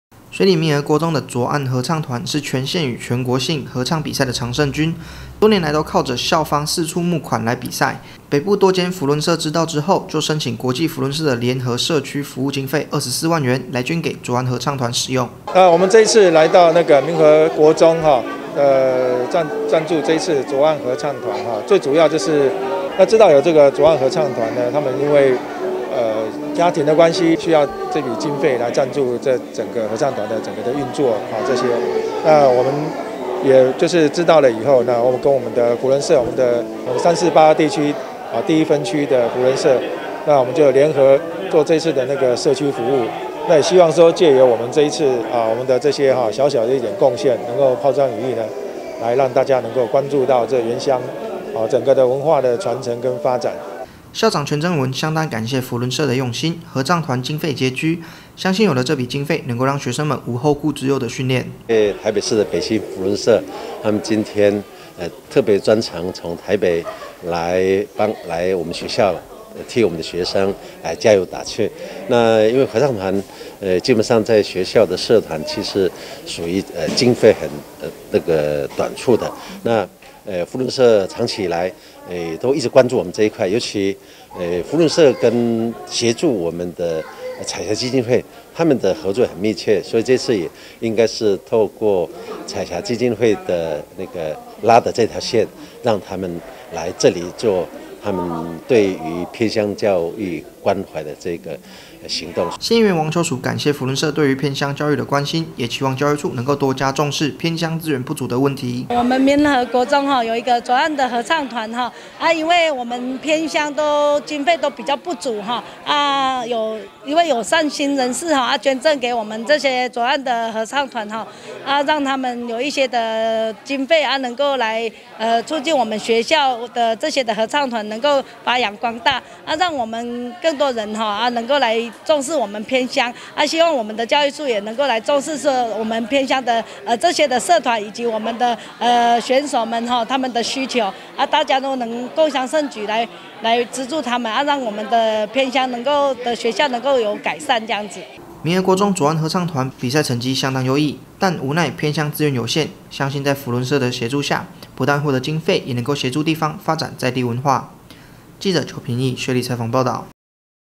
20211013 民議新聞 北部扶輪社捐助24萬 協助民和國中發展藝文教育(縣議員 王秋淑)
水里民和國中的濁岸合唱團，是全縣與全國性合唱比賽的常勝軍，多年來都是靠著校方四處募款維持，北部幾間扶輪社知道後，就申請國際扶輪社的聯合社區服務經費24萬元，捐贈給濁岸合唱團使用，希望與學校共同努力保存這屬於布農族純淨的天籟歌聲。